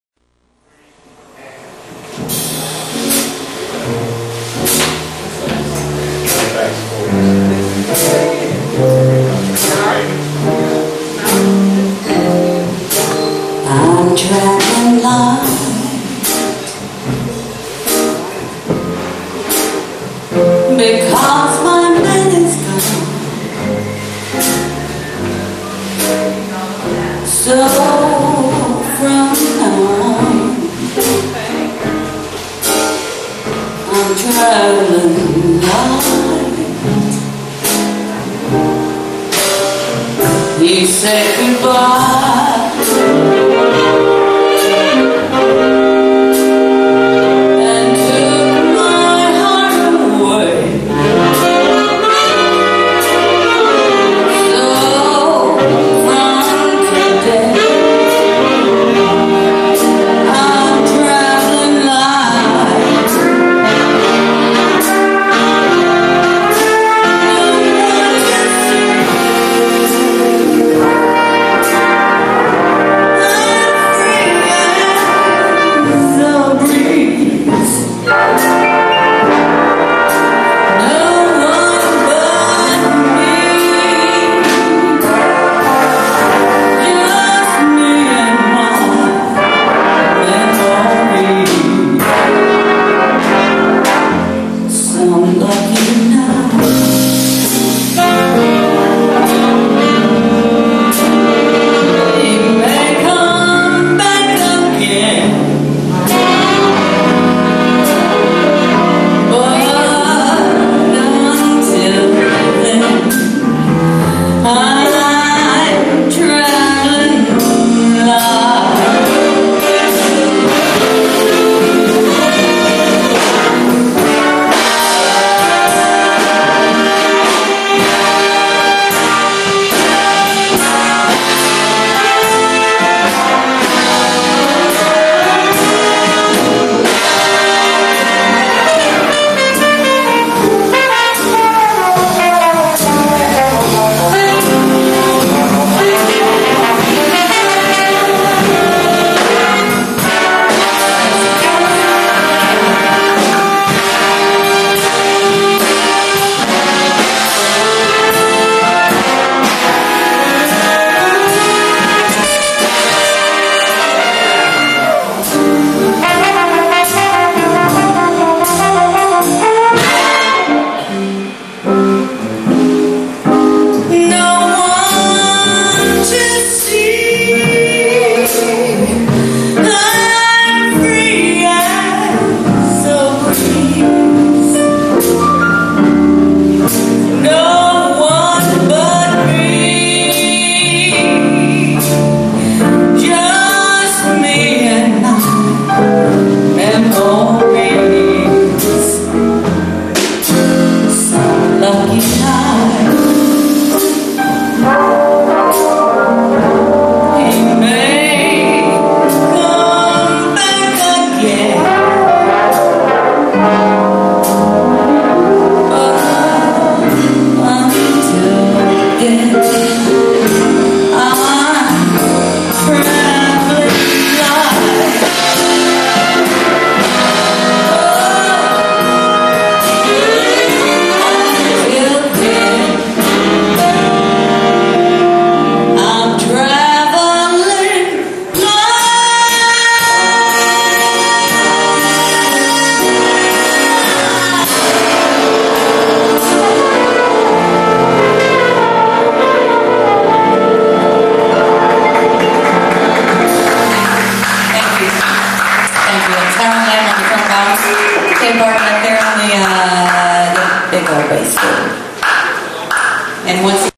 Memphis Jazz Orchestra & Jorge Estrada Avendaño
M.J.O., Jorge Estrada Avendaño: Piano, Teresa Pate: Vocals, Howard Lamb: Trombon & Conduct.